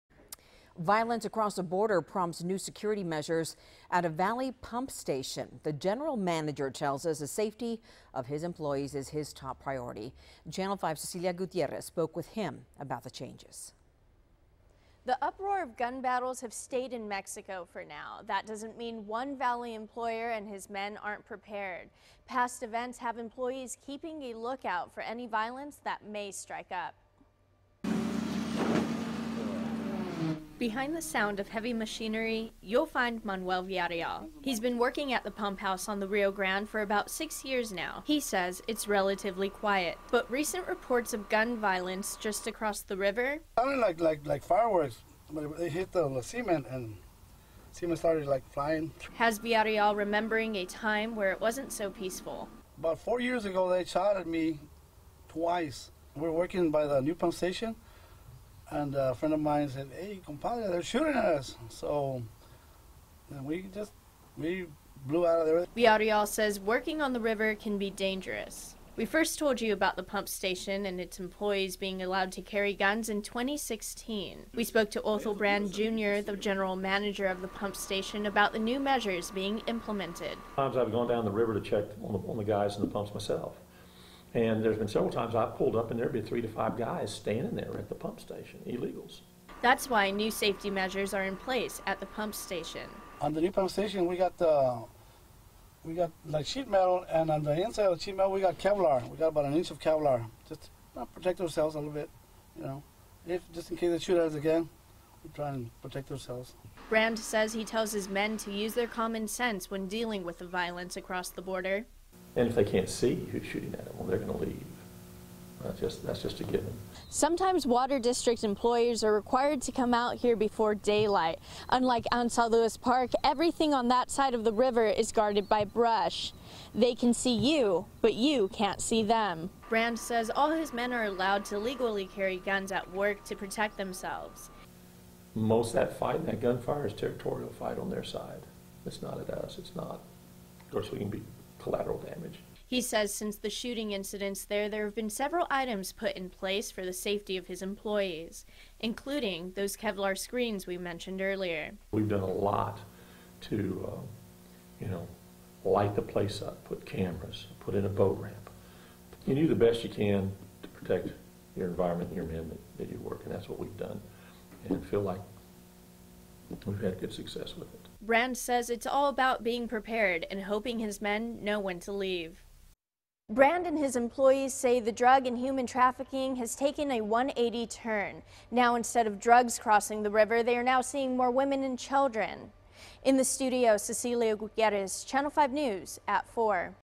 New Safety Measures in Place for Hidalgo Co. Water Pump House
Past events have employees keeping a look out for any violence.